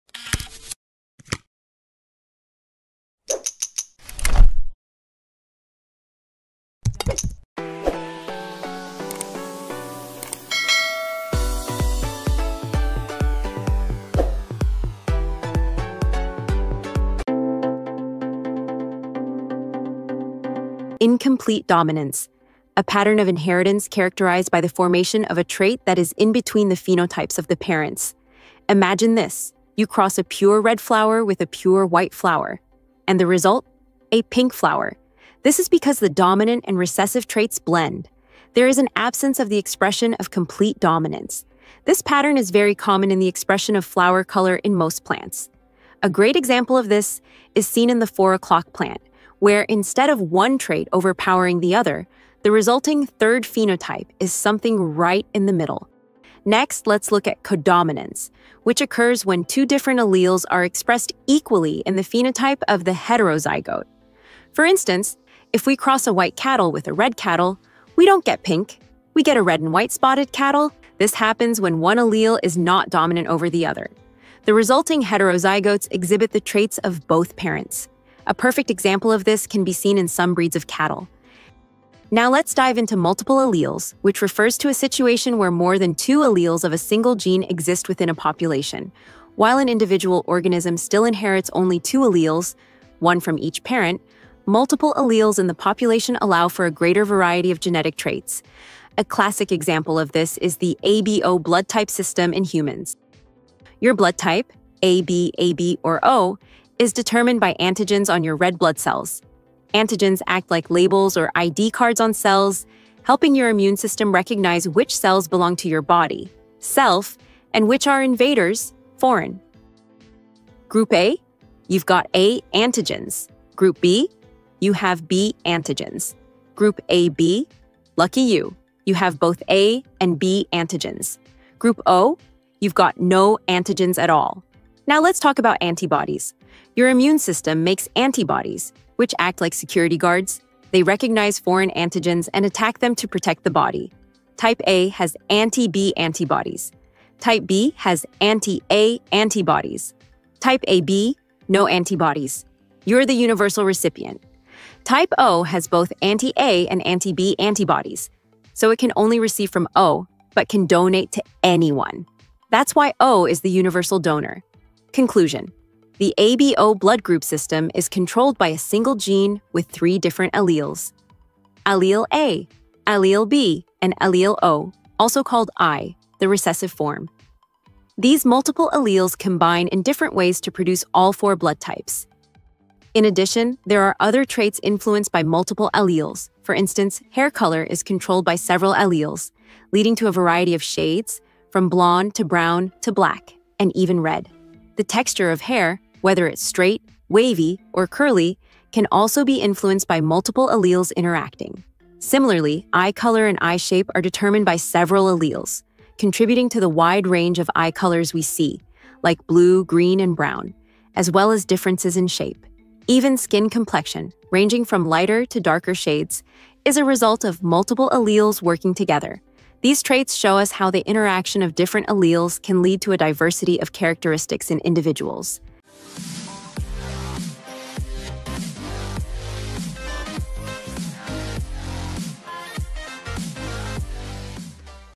Incomplete Dominance, Codominance and Multiple Alleles
Incomplete Dominance
Incomplete Dominance is a pattern of inheritance characterized by the formation of a trait that is in between the phenotypes of the parents.
Imagine this: you cross a pure red flower with a pure white flower... and the result? A pink flower. This is because the dominant and recessive traits blend. There is an absence of the expression of complete dominance.
This pattern is very common in the expression of flower color in most plants. A great example of this is seen in the Four O’clock Plant, where instead of one trait overpowering the other, the resulting third phenotype is something right in the middle.

Codominance
Codominance occurs when two different alleles are expressed equally in the phenotype of the heterozygote.
For instance, if we cross a white cattle with a red cattle, we don't get pink—we get a red and white spotted cattle! This happens when one allele is not dominant over the other. The resulting heterozygotes exhibit the traits of both parents.
A perfect example of this can be seen in some breeds of cattle.

Multiple Alleles
Multiple Alleles refers to a situation where more than two alleles of a single gene exist within a population.
While an individual organism still inherits only two alleles—one from each parent—multiple alleles in the population allow for a greater variety of genetic traits.
A classic example of this is the ABO blood type system in humans.
Your blood type—A, B, AB, or O—is determined by antigens on your red blood cells.
Antigens act like labels or ID cards on cells, helping your immune system recognize which cells belong to your body (self) and which are invaders (foreign).
Group A? You’ve got A antigens.
Group B? You have B antigens.
Group AB? Lucky you—you have both A and B antigens.
Group O? You’ve got no antigens at all!
Now let’s talk about antibodies.
Your immune system makes antibodies, which act like security guards—they recognize foreign antigens and attack them to protect the body.
Type A has anti-B antibodies.
Type B has anti-A antibodies.
Type AB? No antibodies—you’re the universal recipient!
Type O has both anti-A and anti-B antibodies—so it can only receive from O, but can donate to anyone! That’s why O is the universal donor.
Conclusion:
The ABO blood group system is controlled by a single gene with three different alleles:
• Allele A,
• Allele B, and
• Allele O (also called i, the recessive form).
These multiple alleles combine in different ways to produce all four blood types.

In addition, there are other traits influenced by multiple alleles. For instance, hair color is controlled by several alleles, leading to a variety of shades, from blonde to brown to black, and even red. The texture of hair—whether it's straight, wavy, or curly—can also be influenced by multiple alleles interacting. Similarly, eye color and eye shape are determined by several alleles, contributing to the wide range of eye colors we see, like blue, green, and brown, as well as differences in shape. Even skin complexion, ranging from lighter to darker shades, is a result of multiple alleles working together. These traits show us how the interaction of different alleles can lead to a diversity of characteristics in individuals